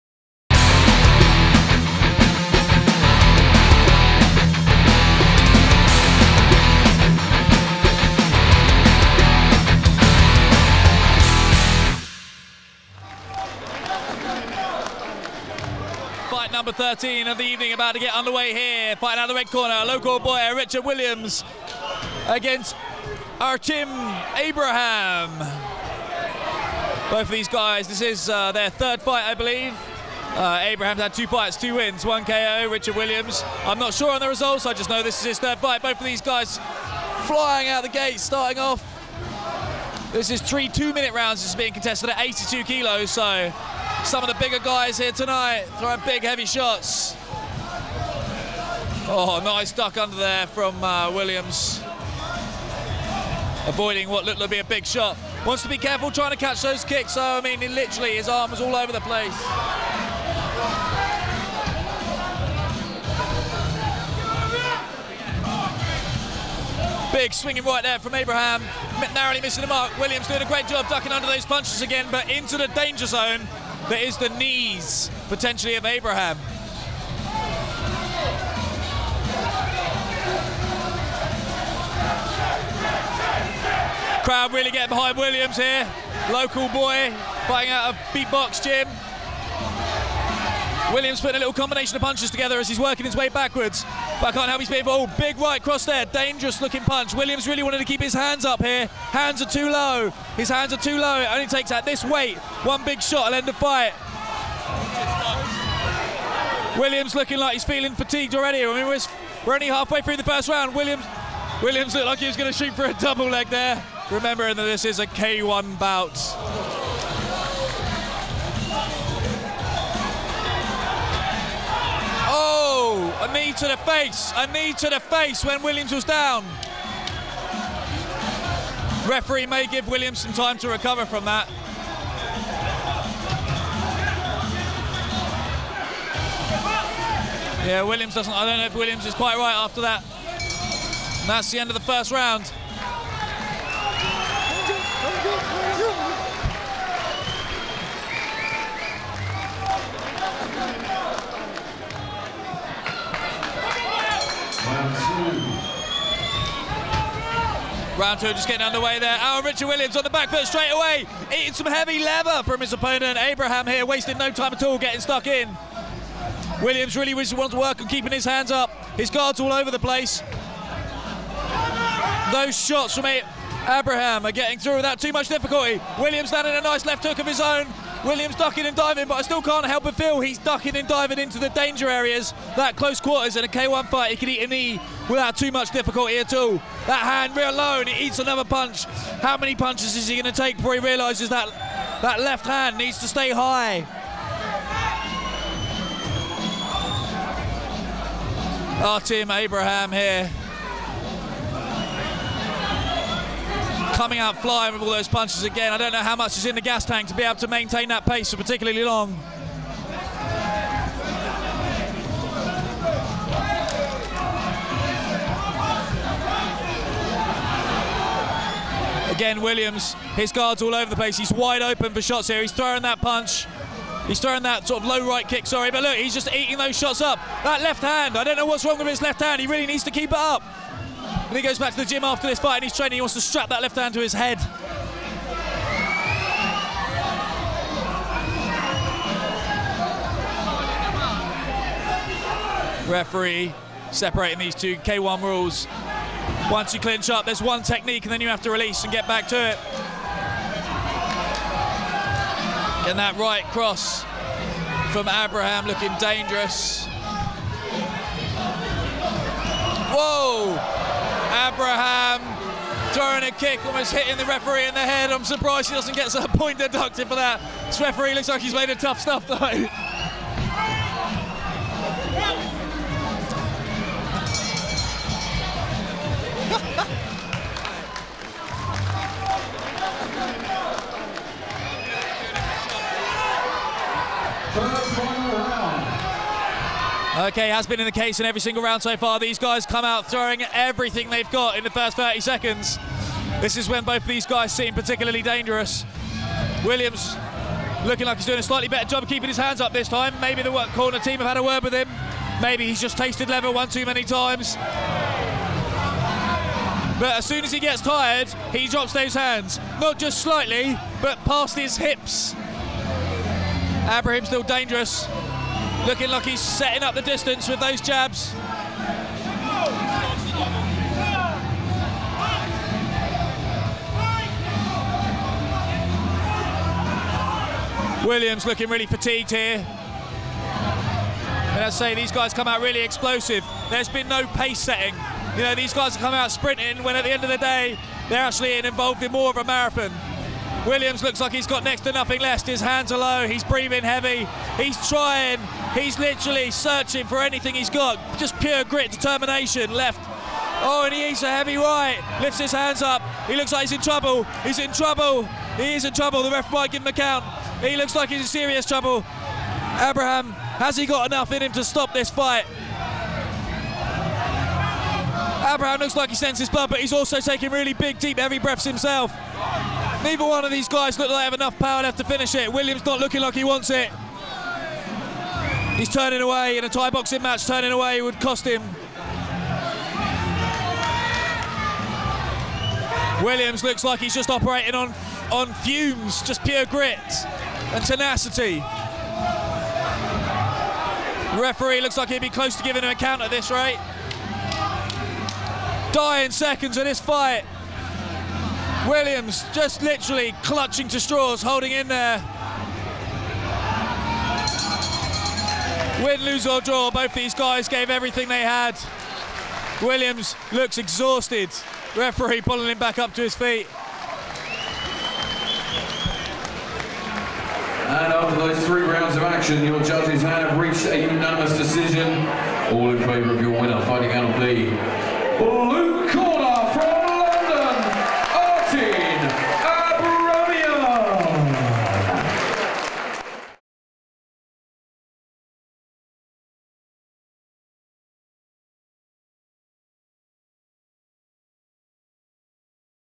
Abriham v Williams - K1. Stand and Bang Oblivion, 28th February 2015
82kg max - K1 - 3x2 minute rounds.

Commentary by Dominic Heydon.